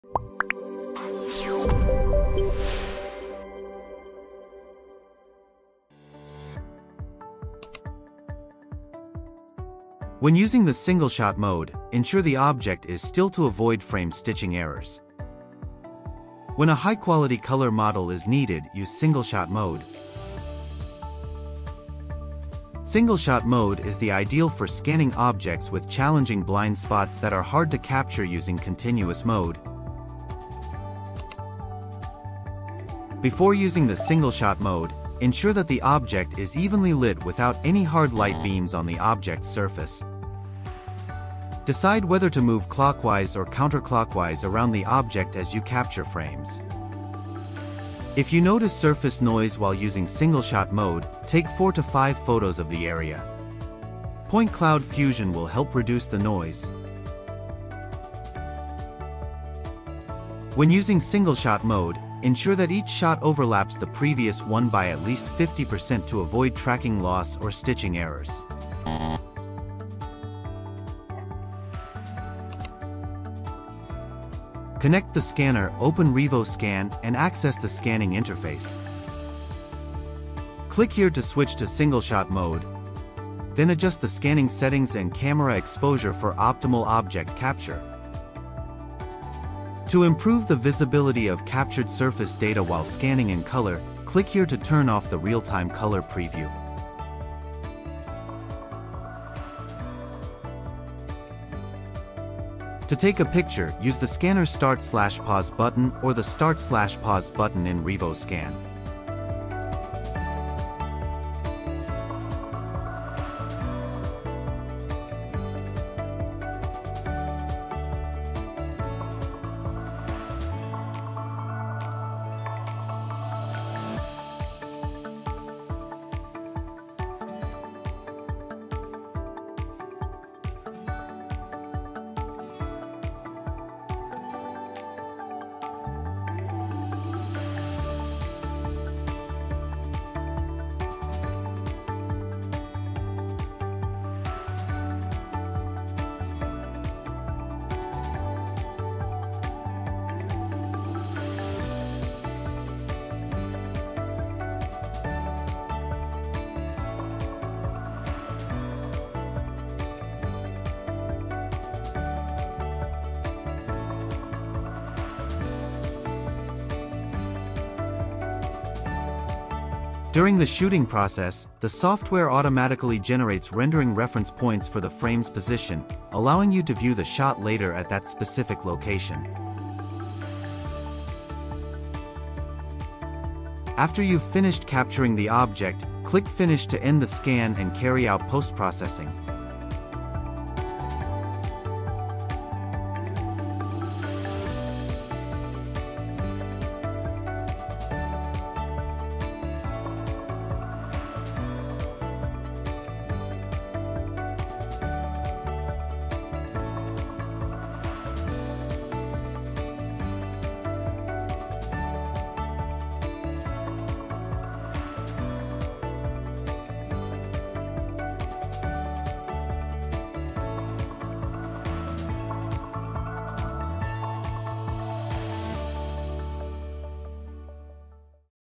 Revo Scan 5: Mastering Single-Shot Mode for Accurate Scans
Learn how to use Single-Shot mode for accurate scans that reduce point cloud overlap and to scan objects with hard-to-capture areas!

P.S. Make sure to download the latest version of Revo Scan 5, or you'll miss out on some of these features.